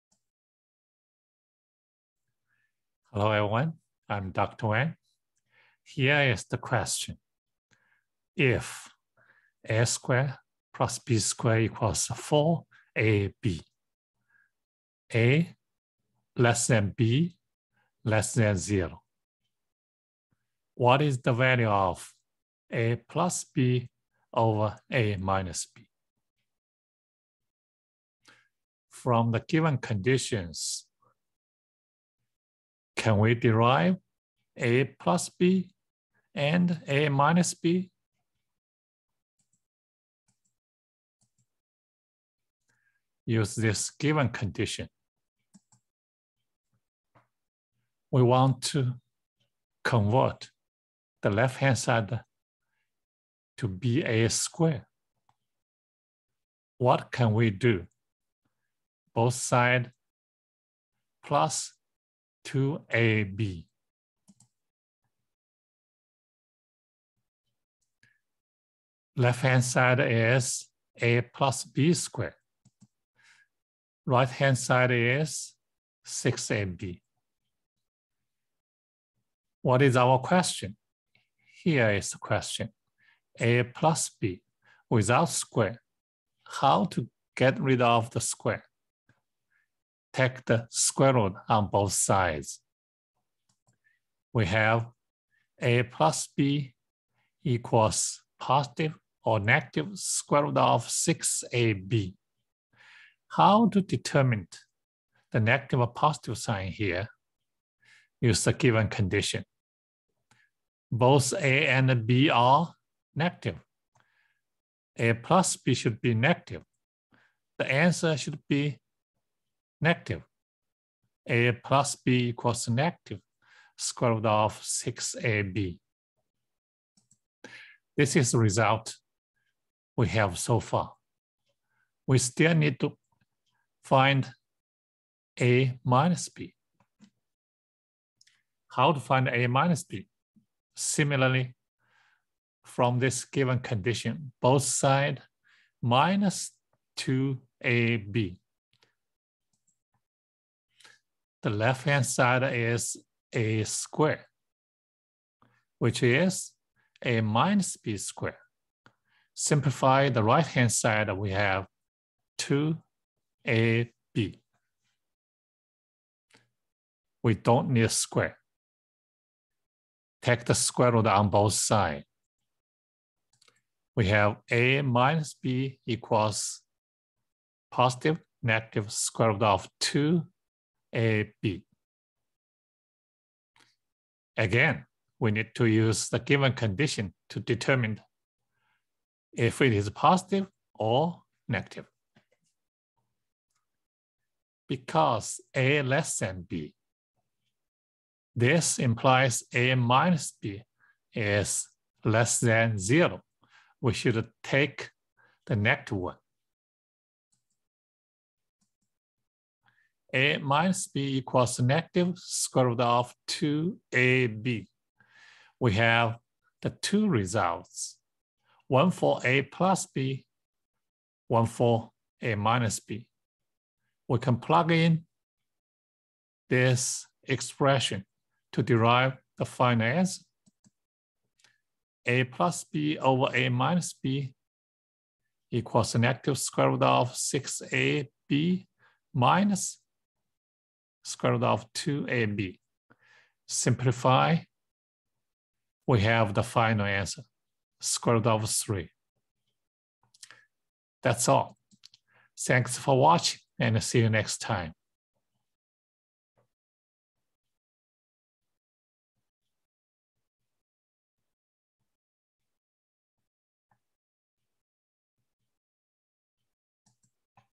(𝒂 +𝒃) / (𝒂 − 𝒃) = ? | Olympiad Question
We use the completing the square method for the given conditions to obtain answers for a+b and
 a-b.